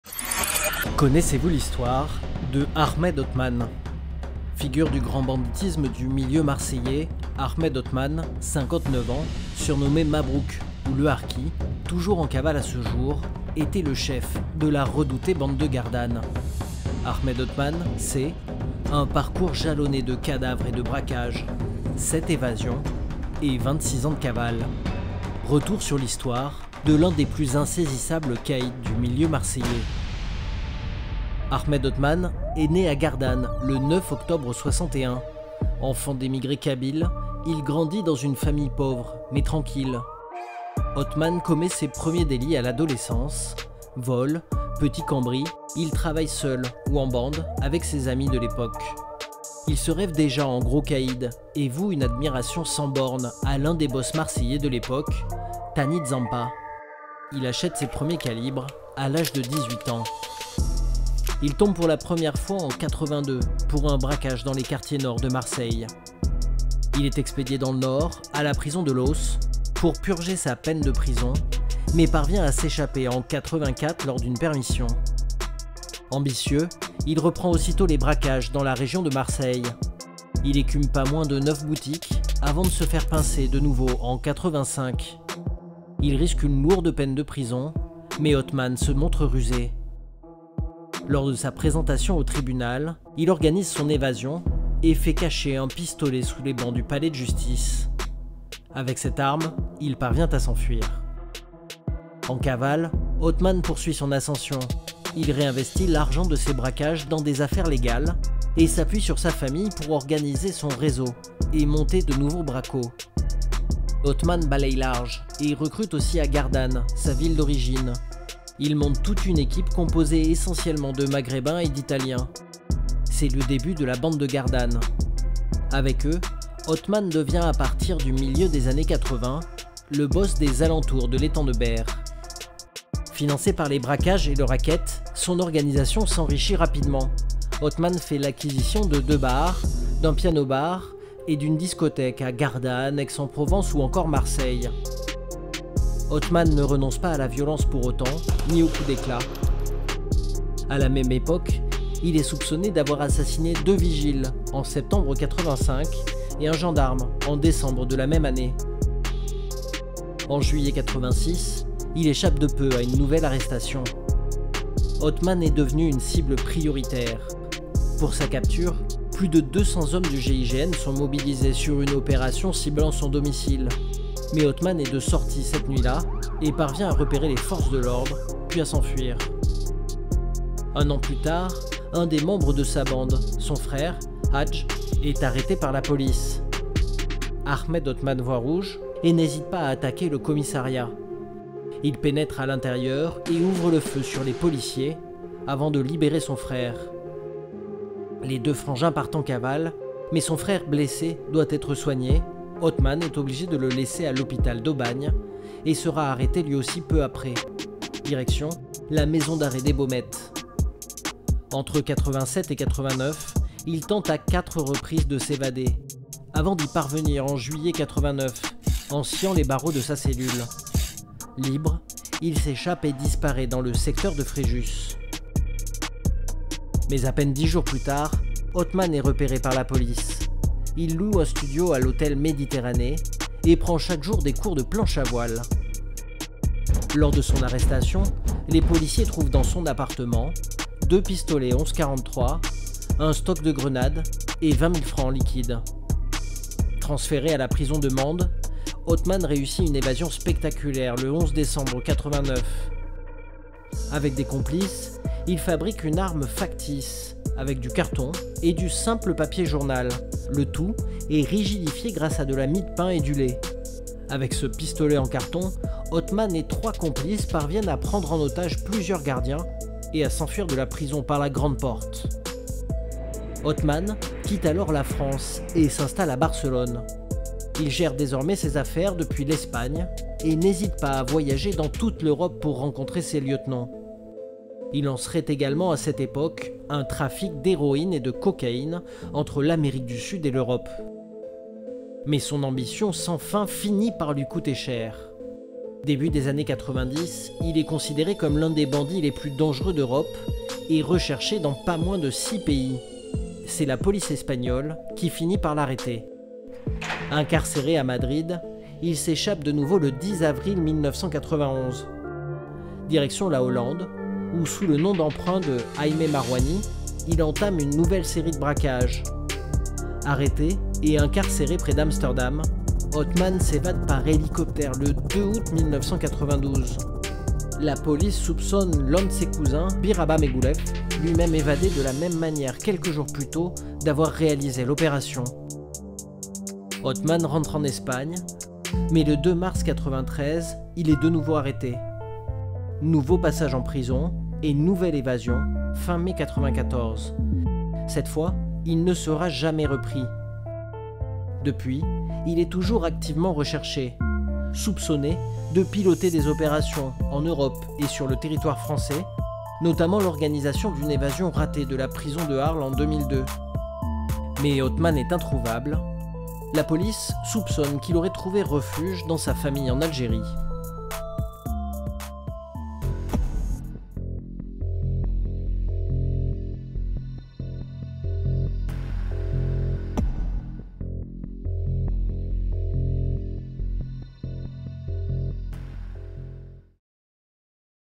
🔥CAÏDS DU 13🔥 - LE MILIEU MARSEILLAIS - 💥💥💥 AHMED OTMANE 💥💥💥
Chaque semaine = de nouvelles vidéos

🔴 Voici l'histoire d'Ahmed Otmane alias "Mabrouk", le chef de la puissante bande de Gardanne.

🟦Rejoins-nous également sur Facebook !